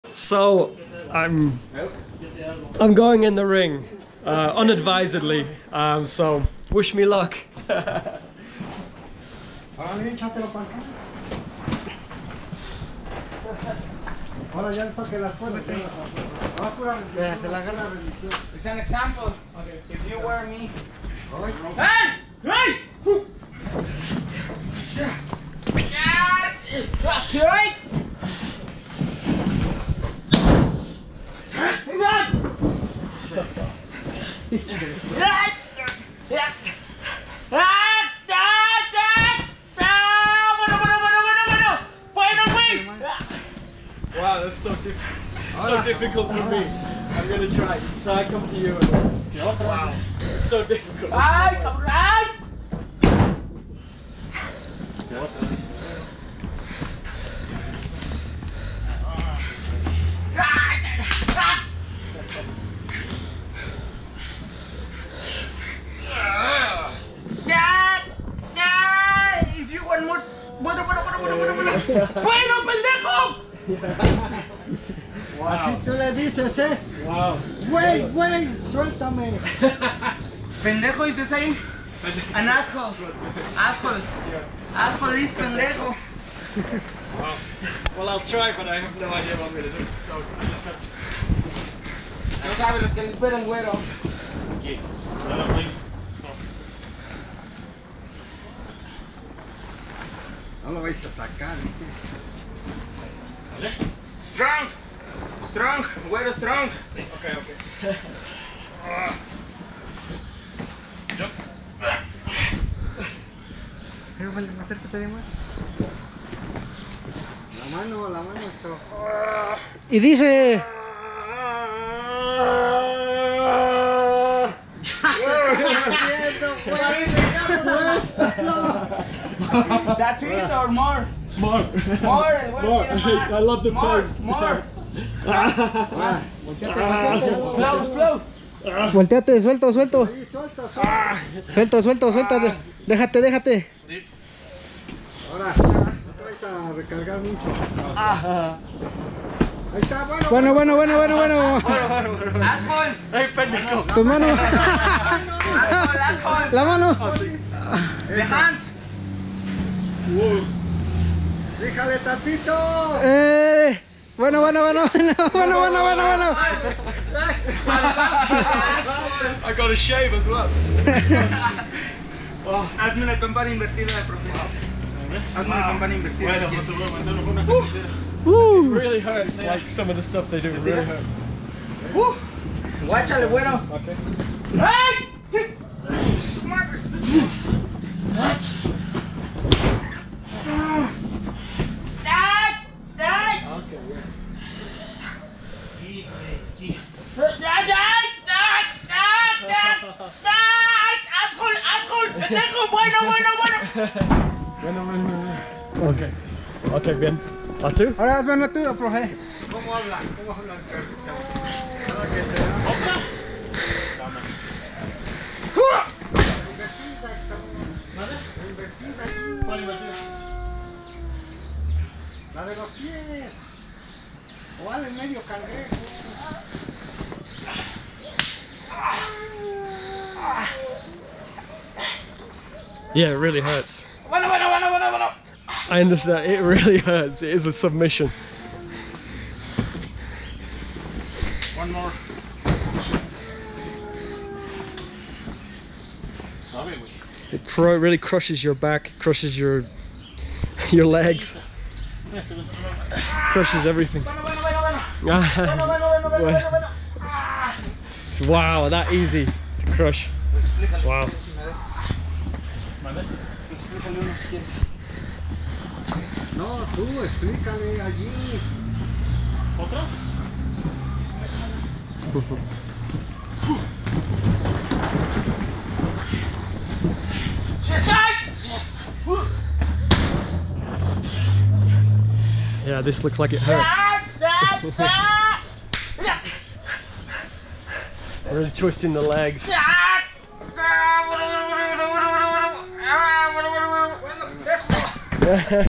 Conor Tezza Gets a Beating - Extreme Lucha Libre Mexican Wrestling in Capulhac Mexico 🇲🇽
Conor Gets a Beating - Extreme Lucha Libre Mexican Wrestling in Capulhac Mexico  🇲🇽 . What do you guys think?   🇲🇽  Slam 🇲🇽
Whilst in the City of Mexico I received a special invitation so I decided to embark on another adventure...